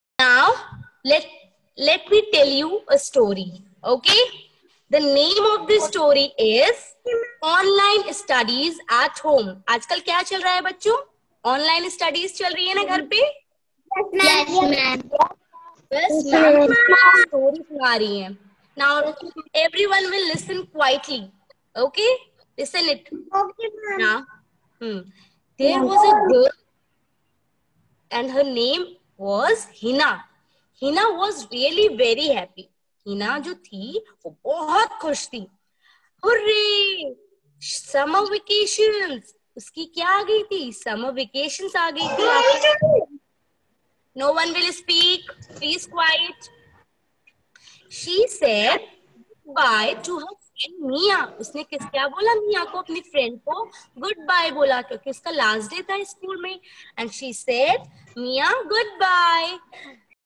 "Story Telling Activity"- "Having a healthy mind is just as important as a healthy body."
"Having a healthy mind is just as important as a healthy body."
The Mental Health Week is observed from 4th to 10th October every year. To observe Mental Health Week Mount Litera Zee School, Roorkee organised a Story telling activity through ZOOM Meeting app by the teachers for the students of Grades-1 to 5 on October 10, 2020, where teachers highlighted the different components of mental well being by the story telling method.